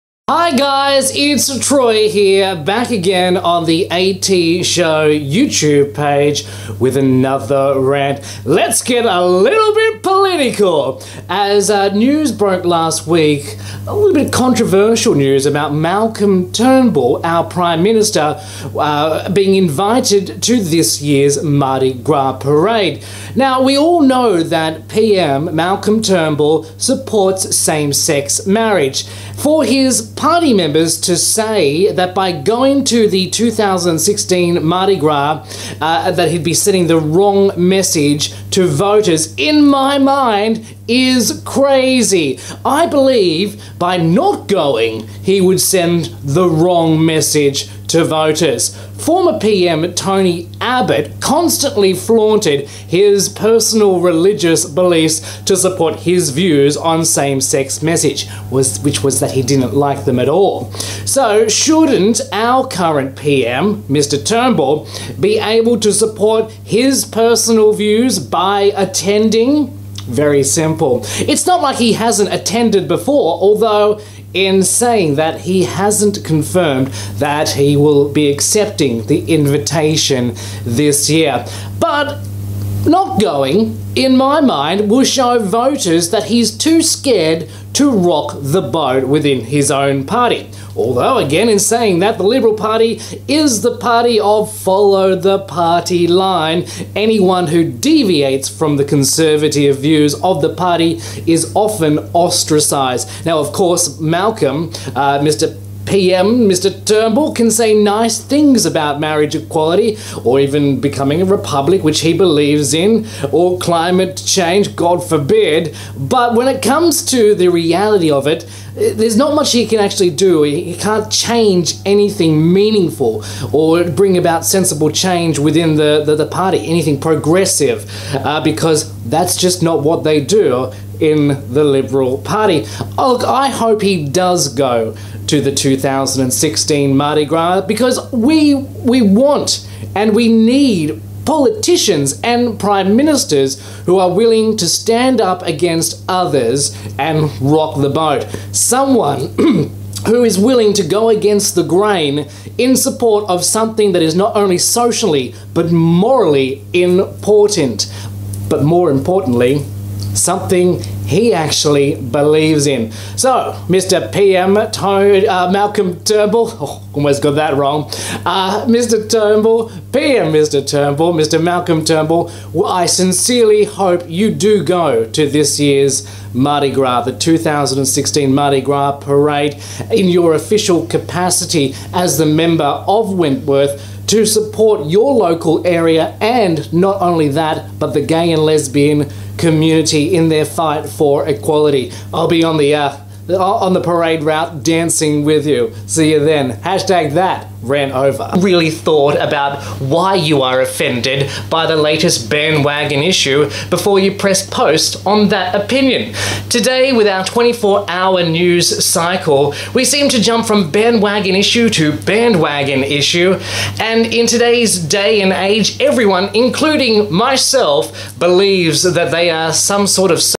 Rocking The Political Boat
Troy has a rant to get off his chest.
Should the Australian Prime Minister Malcolm Turnbull​ go to Sydney Mardi Gras​ 2016? If he doesn't, what message will it sending voters?